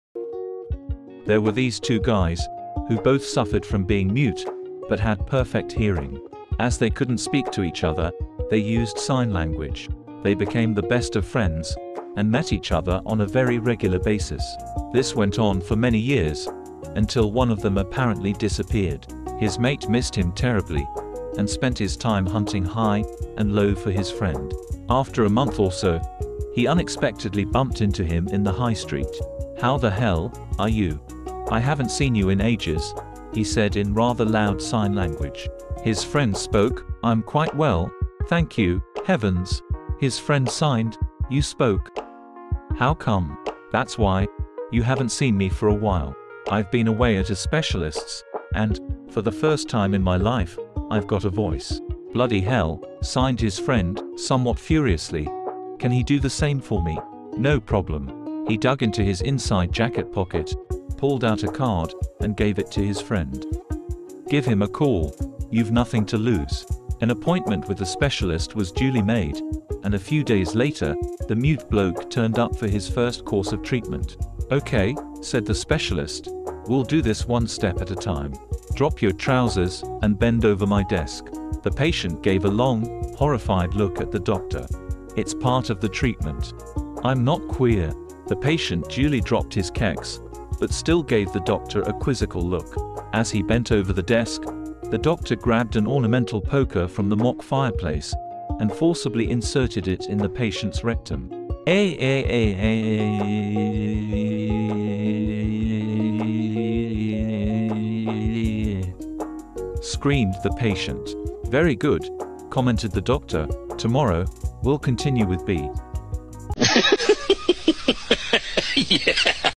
🤣❤️ We Love Jokes: There were two blokes, who both suffered from being mutes...!
THE STORY:
There were two blokes, who both suffered from being mute, but had perfect hearing.
As they couldn’t speak to each other, they used sign language.
They became the best of friends, and met each other on a very regular basis.
This went on for many years, until one of them apparently disappeared.
His mate missed him terribly, and spent his time hunting high, and low for his friend...